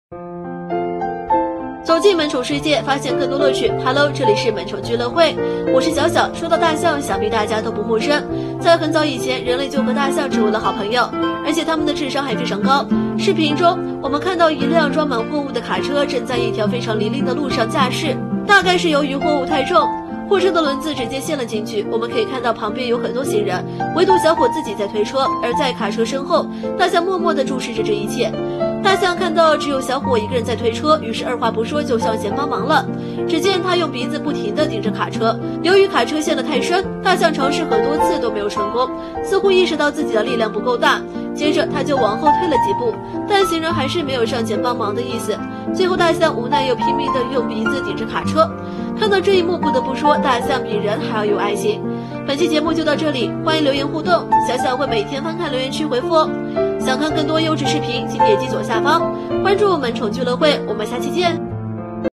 卡车深陷泥潭无法行驶，大象主动上前帮忙，霸气开始了！
卡车深陷泥潭无法行驶，大象主动上前帮忙，霸气开始了！,本视频由科技聚乐会提供,5153次播放,好看视频是由百度团队打造的集内涵和颜值于一身的专业短视频聚合平台